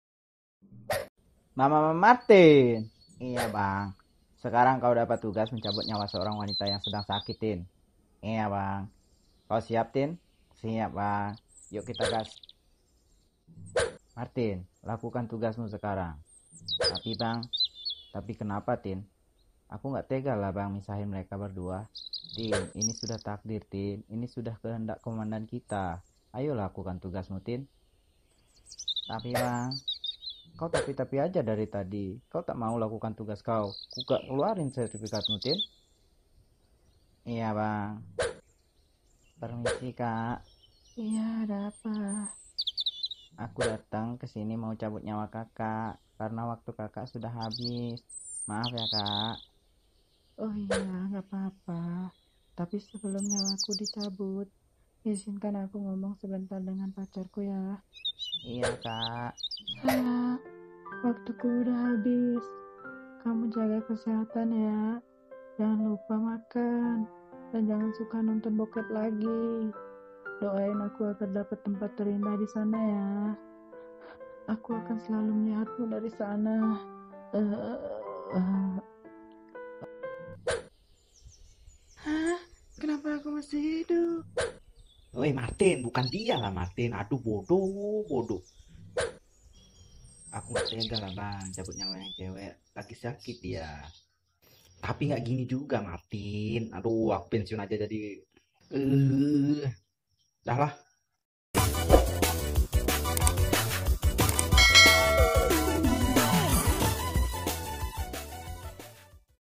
kartun lucu indonesia - martim si malaikat magang dari @Vernalta tidak tega mencabut nyawa
kartun lucu indonesia - martim si malaikat magang dari @Vernalta tidak tega mencabut nyawa Martin si malaikat magang tidak tega mencabut nyawa seorang wanita

cerita ini hanya hiburan semata,
semoga menyimak video ini di beri
 kesehatan,

dan untuk yang mw dukung Chanel ini silahkan tekan tombol subscribenya makasih